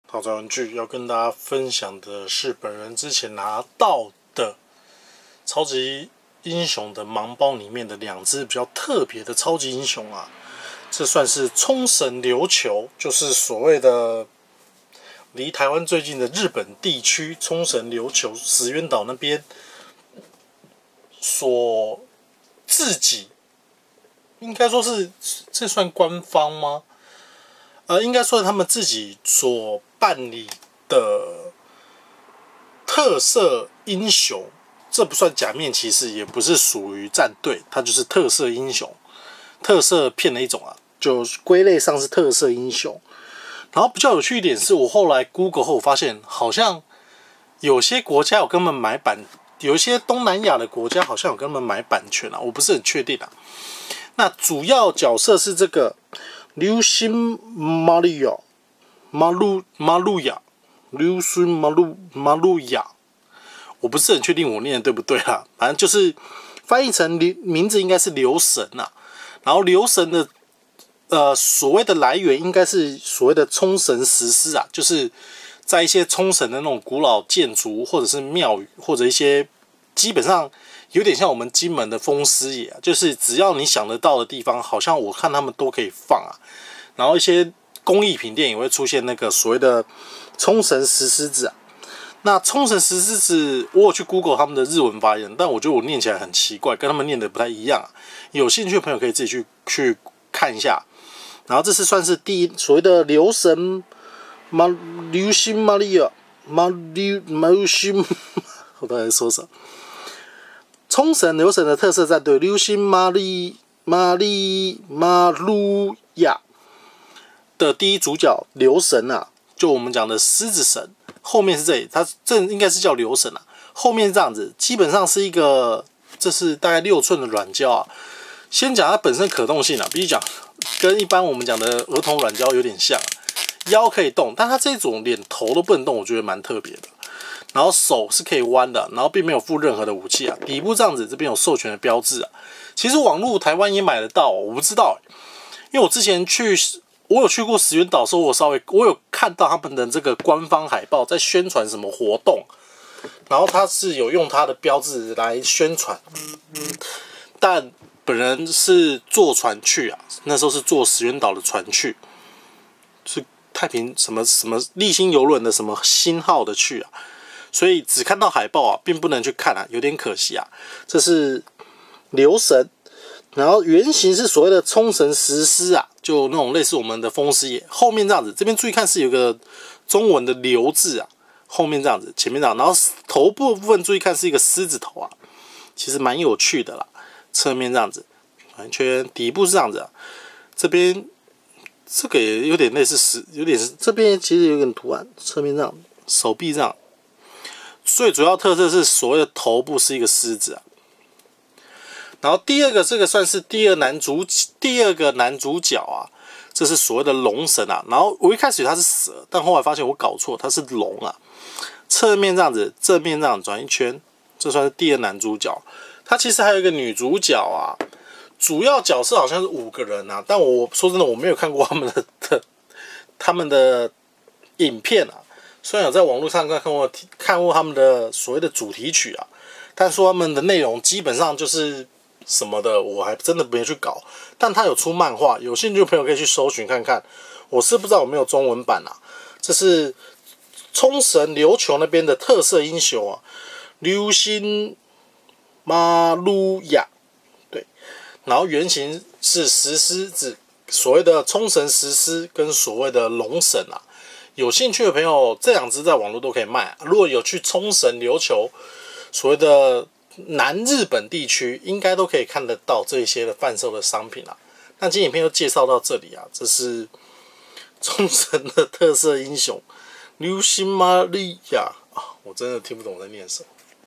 豪宅玩具~1792~(原諒我的奇怪發音)日本沖繩琉球區域限定 特攝英雄 琉神Mabuya 海龍 軟膠公仔
2008年日本沖繩琉球區域開始拍賣的限定 特攝英雄 琉神Mabuya
特色除了是區域自己開發的特攝英雄以外!
滿滿的南日本風個造型(沖繩石獅)
另外就是它故事設定好像基本只會阻止敵人做惡!並沒有打死敵人!
這個設定真的算滿特別的
好像在當地也會做一些活動宣傳!
也有舞台劇表演!
有去沖繩琉球的朋友可以留意看看還有沒有相關的活動